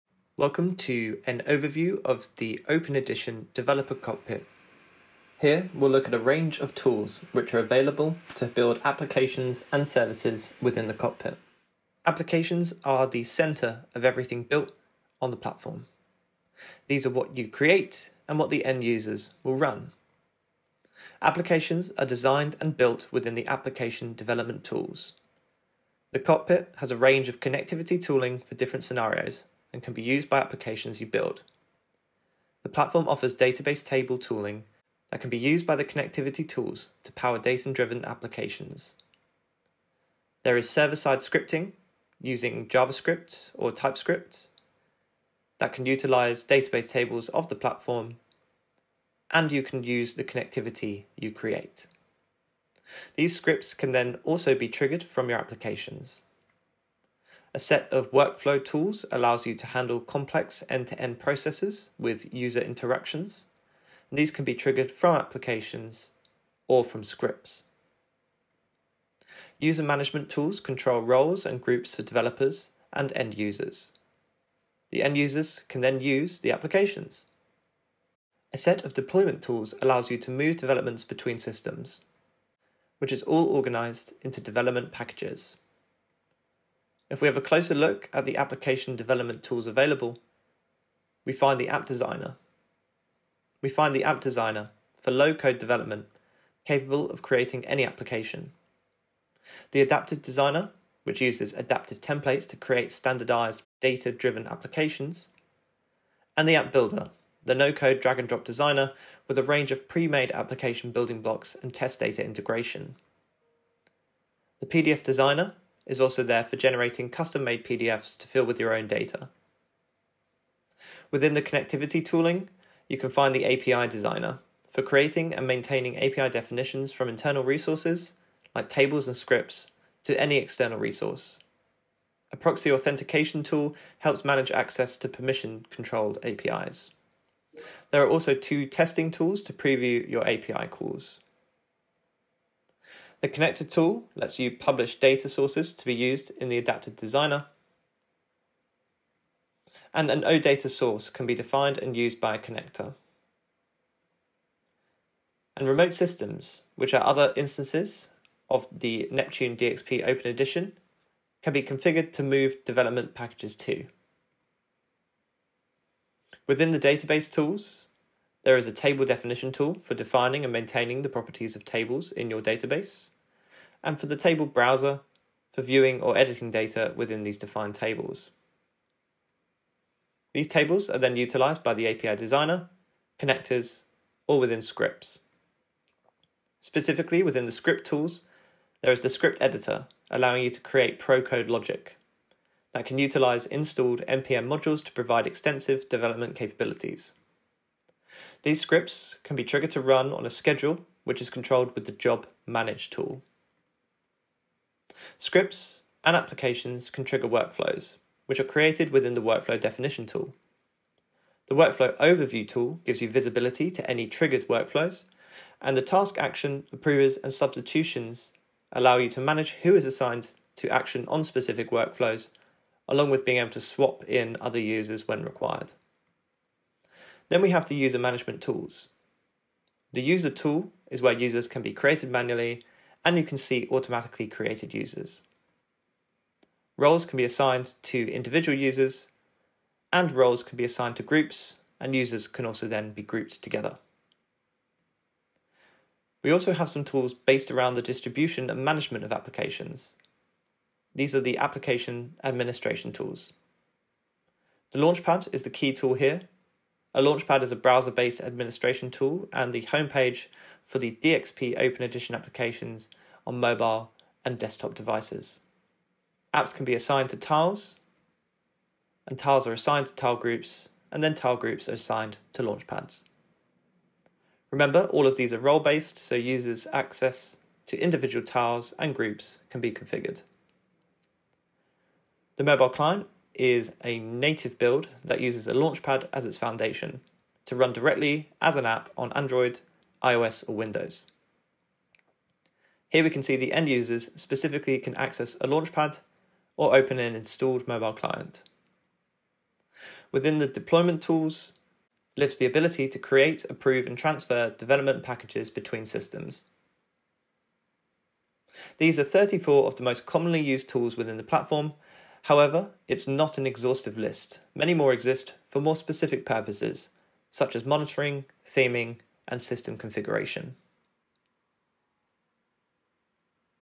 Neptune Software - Open Edition Developer Cockpit Tool Overview
Watch and learn about how the Application Development, Connectivity, Database, Scripting, Workflow, User Management and Deployment tooling work together - with a high-level overview of 34 of the key cockpit tools used in the Neptune DXP Open Edition.

The tools featured:

Development Package
App Builder
Adaptive Designer
App Designer
PDF Designer
Connector
OData Source
Remote Systems
Swagger UI
API Client
API Designer
Proxy Authentication
Table Definition
Table Browser
Script Editor
Job Manage
NPM Modules
Workflow Definition
Workflow Overview
Workflow Task Action
Workflow Approvers
Workflow Substitutions
User
Security Group
Role
Tile
Tile Group
Launchpad
Mobile Client
Deployment Create
Deployment Approve
Deployment Transfer
Deployment Routes
Deployment Log